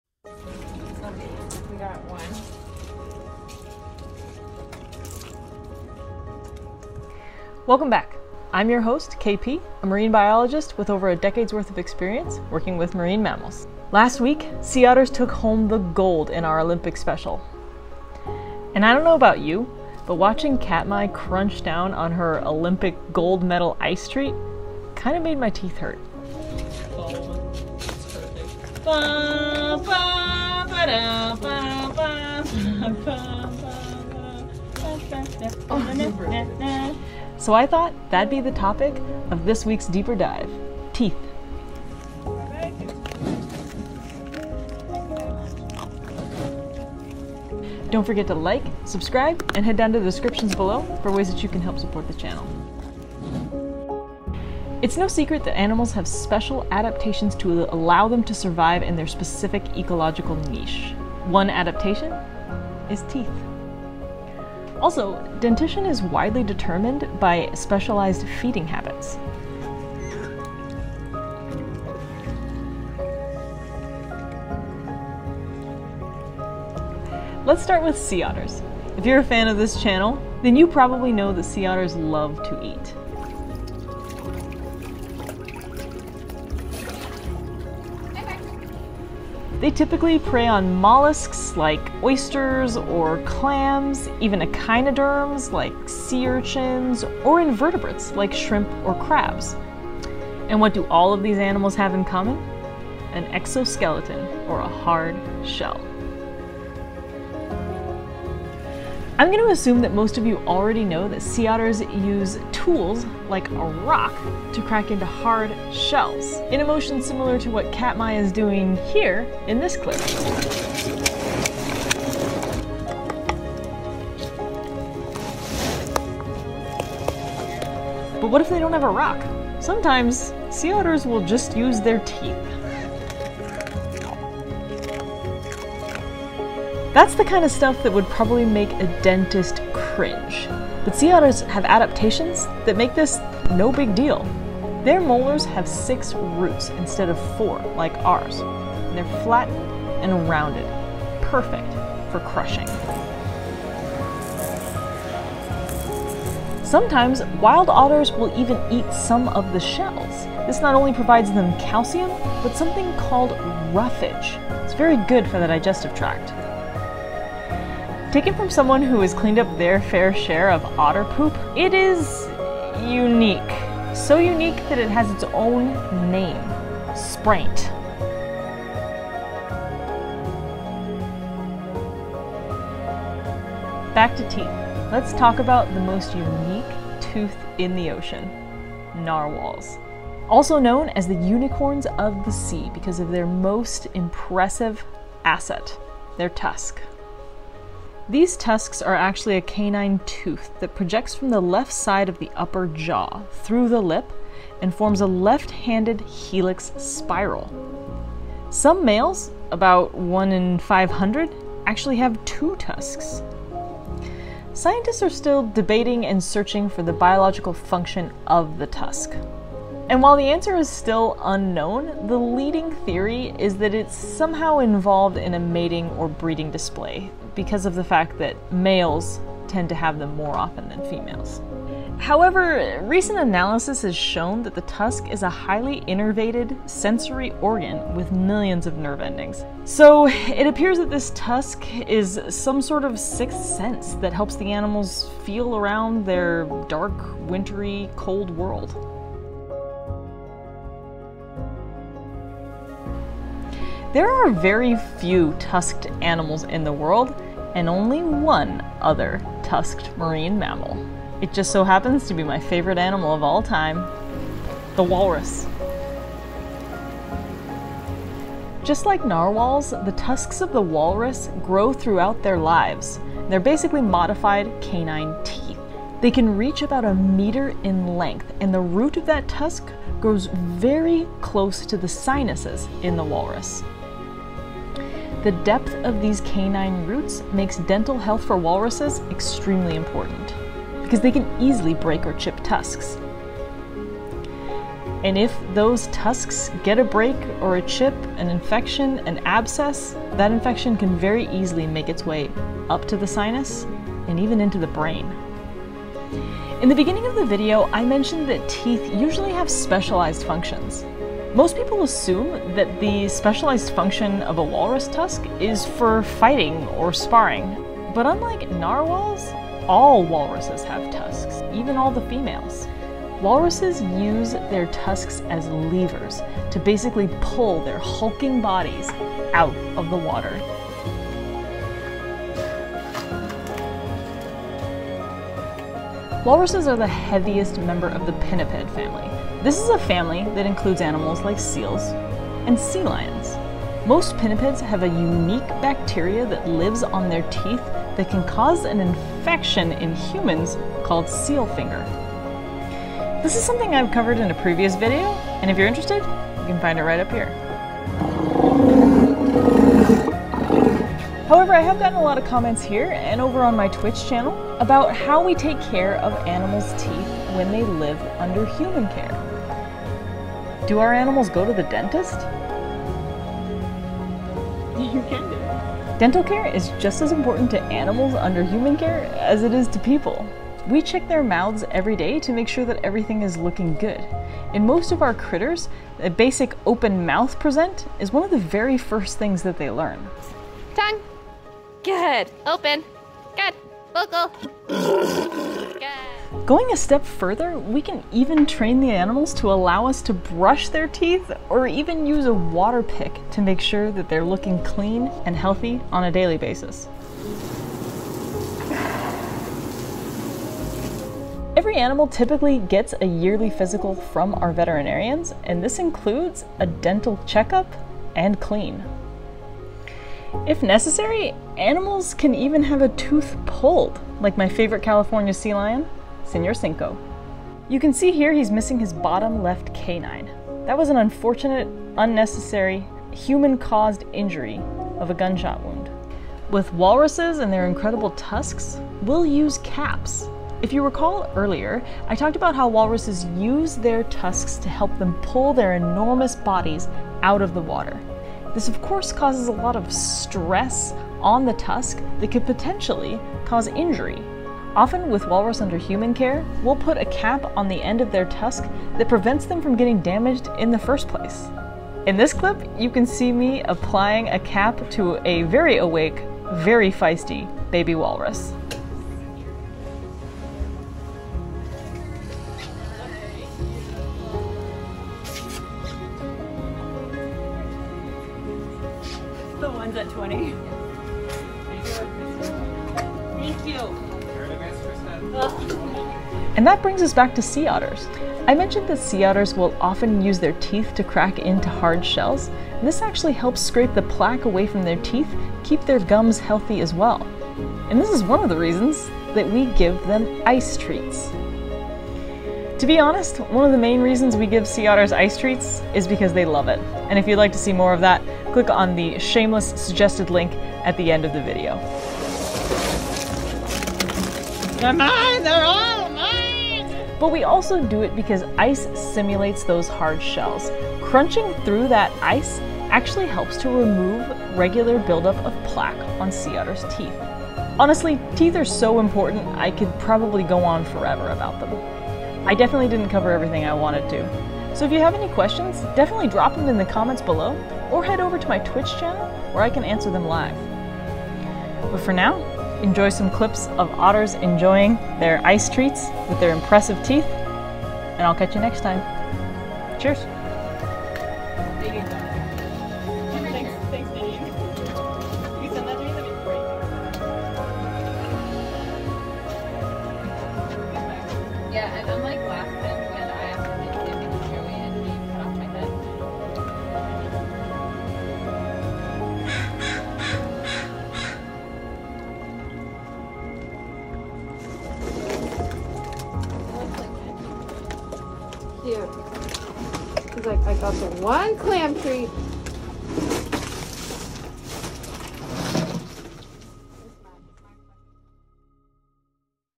Sea Otters LOVE Ice! But Why -- A Marine Biologist EXPLAINS
Teeth are important for every creature that has them! From the long spear of the narwhal to the crushing of the sea otter, they are all specially designed for the needs of the animals!

In the wild, sea otters will often eat the shells of clams, mussels, and sea urchins. These rough shells help keep their teeth clean by scraping away plaque and debris. Unfortunately, it's not always possible to give the shells to sea otters under human care. This is because we carefully examine the quality and quantity of their food. Something that isn't always possible with shellfish unless the shells have been removed. That's one of the reason we give the sea otters these ice treats! Chewing on ice simulates the hard shells. It helps keep their teeth clean! Plus, ice helps these otters stay cool during the warm summer months.

All the otters in this video were rescued when they were extremely young after being abandoned or separated from their moms. After their rehabilitation, federal agencies like the NOAA in the US or the Department of Fisheries Canada, deemed them non-releasable due to the fact that they lack the skills to survive in the wild.
Come chat with me live and ask your marine biology questions
MUSIC FROM
Scott Buckley

NARWHAL IMAGES COURTESY OF:
National Oceanic and Atmospheric Administration NOAA,
Office of Ocean Exploration and Research
Taken or made as part of an employee's official duties.